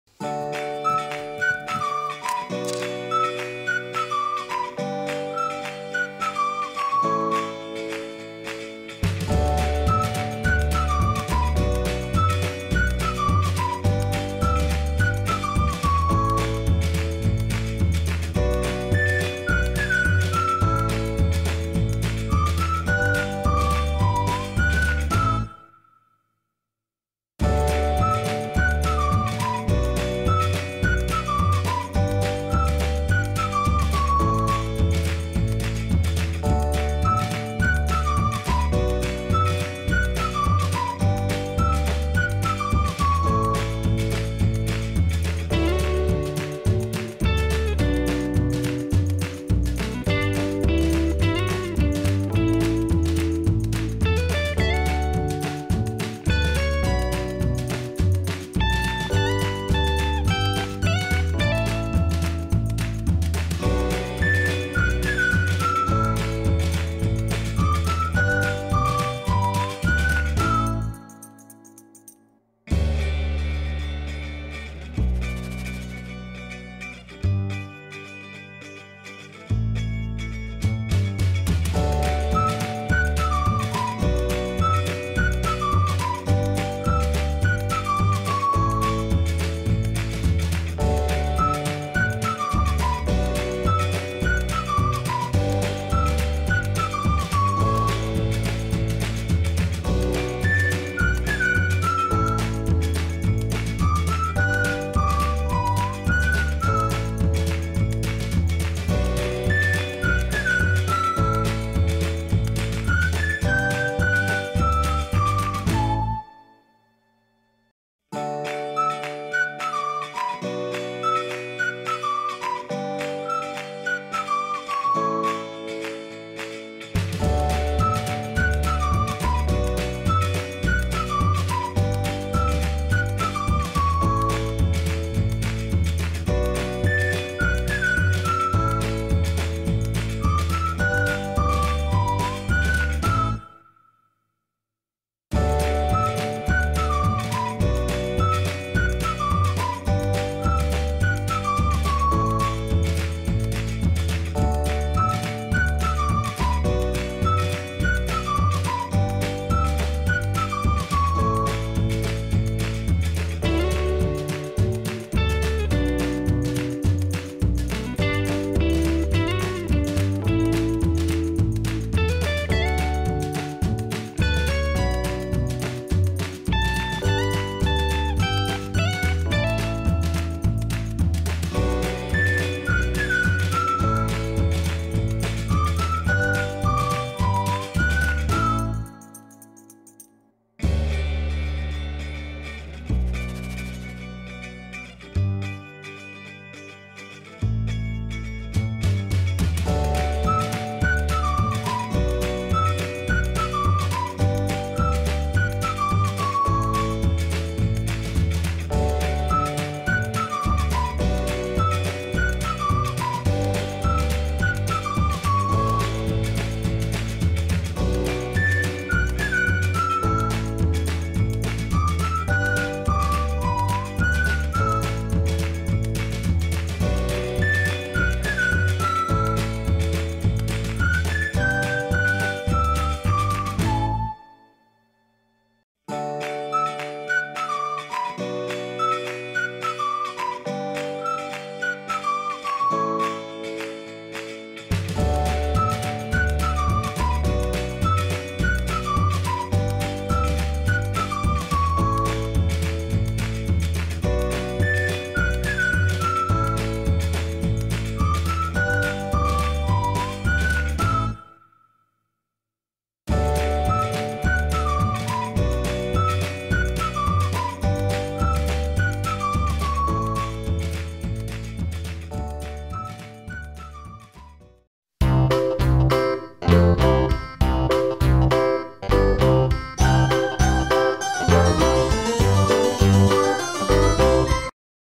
How to turn words SPIKE（Dark lord spike｜Brawl Stars）into a cartoon - How to draw doodle art on paper
brawlstars # howtodraw
Today's video is 「How to turn words SPIKE（Dark lord spike｜Brawl Stars）into a cartoon - How to draw doodle art on paper step by step.」

Let's learn "how to traditional drawing" "3D drawing" "easy paper craft" and so on from this YouTube channel.